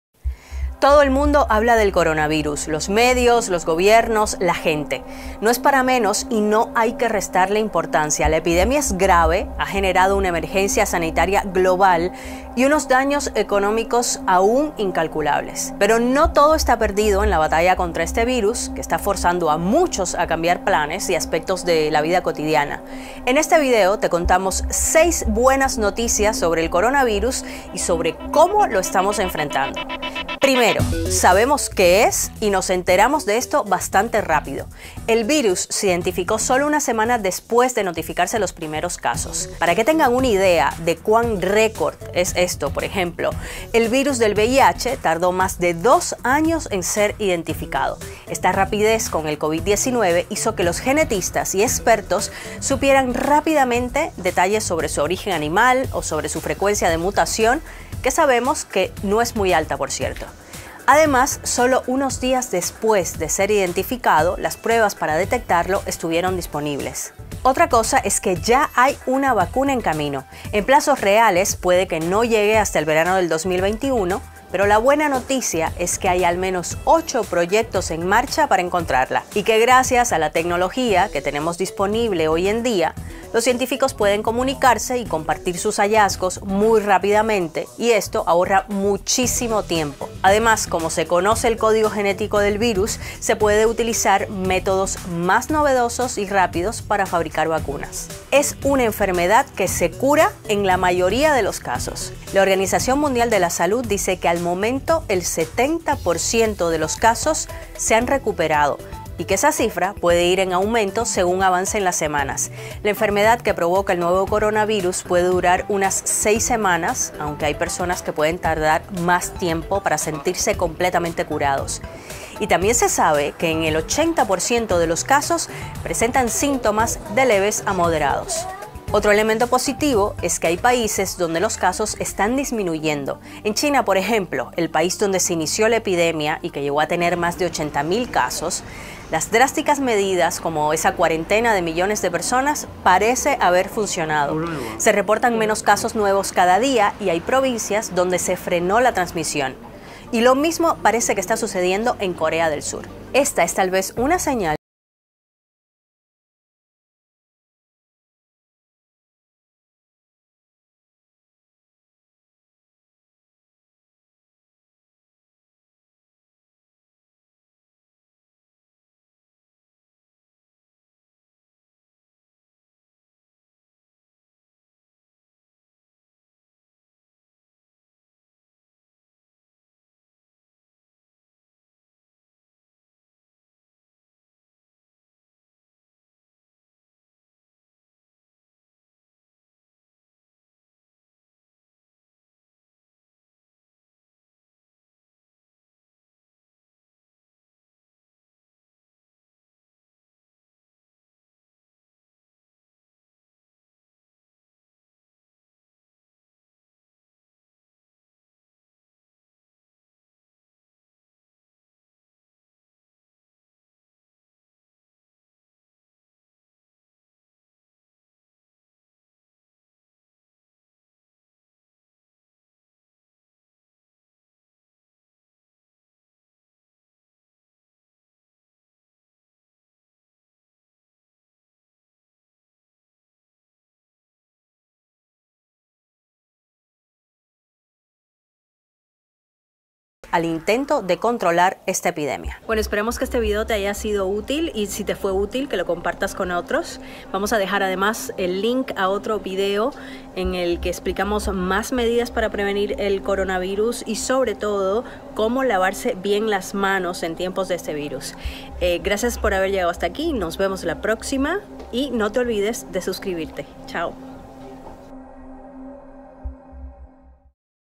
Coronavirus: 6 Buenas Noticias Sobre el Nuevo Virus Covid-19 | BBC Mundo
La epidemia causada por el nuevo coronavirus es grave y ha generado una emergencia sanitaria global. Pero no todo son malas noticias en la batalla contra este virus.

● Presentación y guion: Ana María Roura
● Edición de video: Enric Botella
● Editora: Natalia Pianzola

Healthy lifestyle is the premier place for how to make the most out of your fitness routine, Diet,Travel,Family and Fragrances.